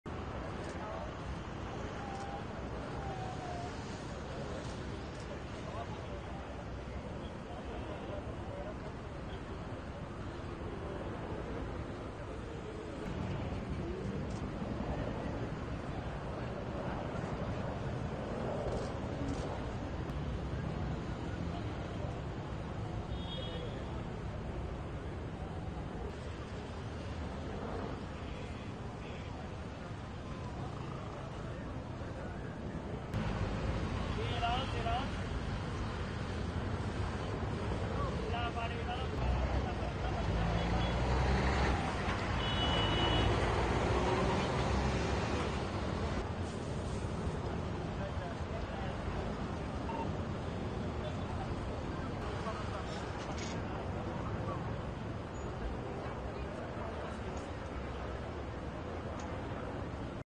Relatives gather at the scene of apparent car bomb in Bogota
Relatives gather at the scene an apparent car bomb attack on a police cadet training school in the Colombian capital that has left at least eight people dead and 10 injured, the defense ministry said. IMAGES